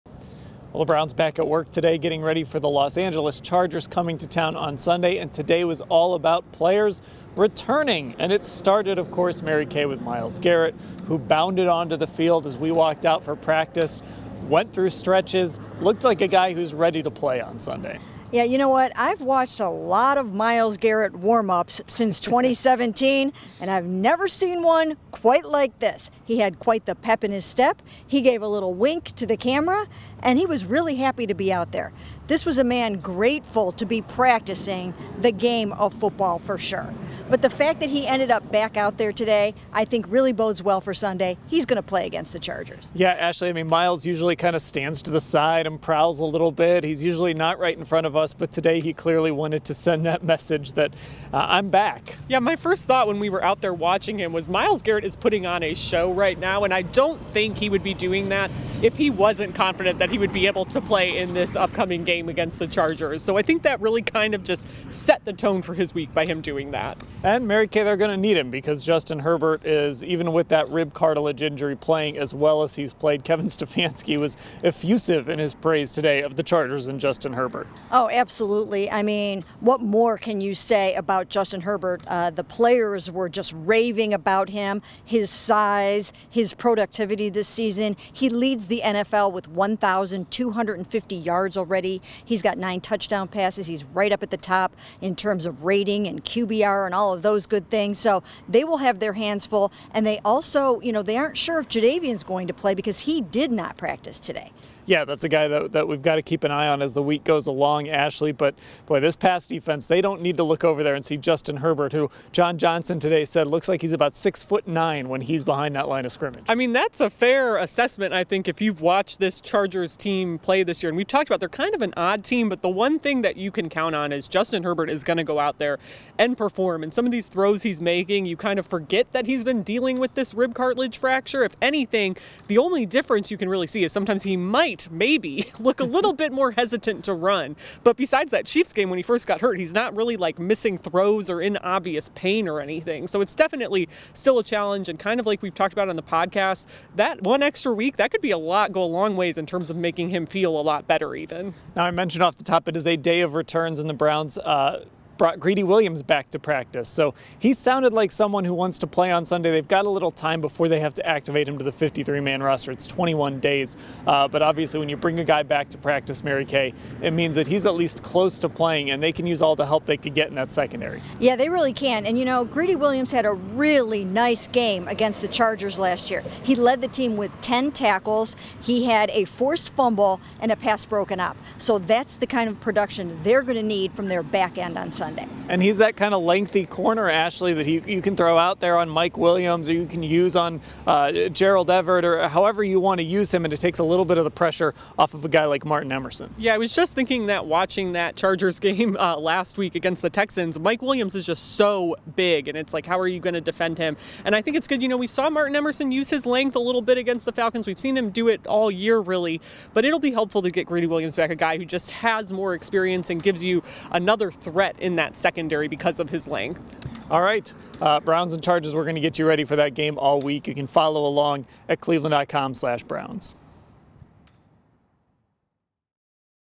Myles Garrett, Greedy Williams return to Browns practice: Berea report
Cleveland.com’s Mary Kay Cabot, Dan Labbe and Ashley Bastock share what they took away from Cleveland Browns practice as the team prepares to face the Los Angeles Chargers on Sunday at FirstEnergy Stadium. Wednesday, Oct. 5, 2022